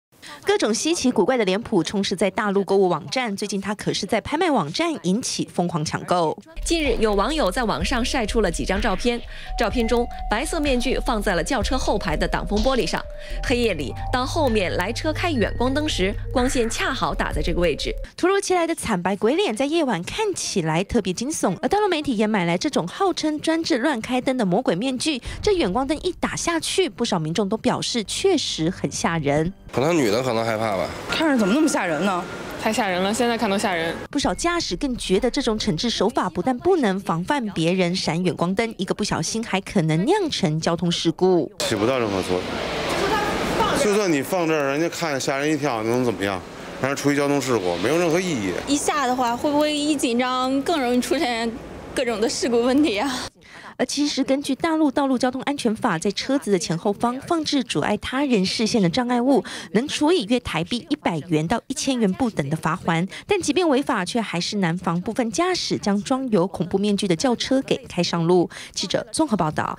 20150712中天新聞 專治亂閃遠光燈？陸賣「鬼面具」嚇駕駛
不少駕駛都曾遇過，在夜間亂開遠光燈的車輛，近日大陸興起一種方法，專門對付這種情況，把「鬼臉面具」放在後擋風玻璃，這樣後方來車閃遠光燈時，「鬼臉」就會出現，但這樣的做法也引發爭議，認為用「嚇人」的方式，一個不小心，可能會釀成交通意外。